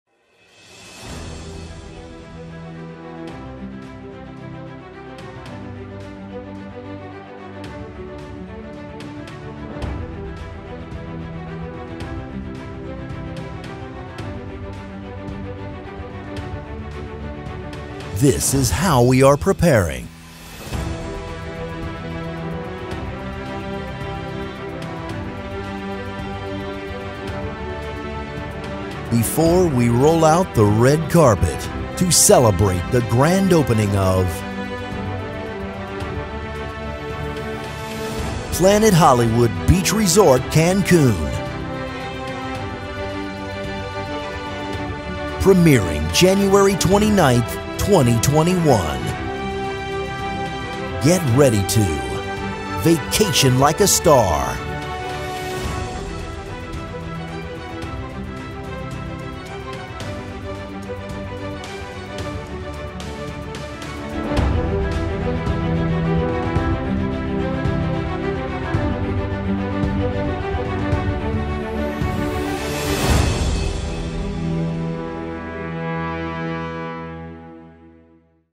Vacation Like a Star at Planet Hollywood Cancun!
Planet Hollywood Beach Resort Cancun is bringing big screen excitement to the pristine shores of Playa Mujeres. Travellers can Vacation Like a Star™ at this entertainment-themed resort, offering a range of amenities and activities for A-listers of all ages; plus authentic Hollywood memorabilia located around the property. Guests who love hanging out at the pool can float down the lazy river or zoom down one of the exhilarating water slides at the on-site water park and splash pad. There’s plenty of excitement on land as well, whether vacationers want to channel their inner Happy Gilmore at the mini golf course or visit the unique Planet Play with an indoor trampoline park (at an extra cost). Mini stars and starlets will have a blast at the kids club with dance parties and awards ceremonies, while parents can treat themselves to some much-deserved relaxation time at the PH Spa. After a day spent living the VIP life in paradise, guests can grab a bite to eat at one of the resort’s restaurants, featuring a range of delectable options including celebrity chef Guy Fieri’s Burger Joint. Whether vacationers are travelling as a couple or with the entire entourage, accommodation options are aplenty with lavish amenities such as 24-hour room service and PluggedIn™ connectivity. Plus, for extra luxury, guests can upgrade to Star Class™ for their own personal agent, a resort rider, exclusive lounge access and more.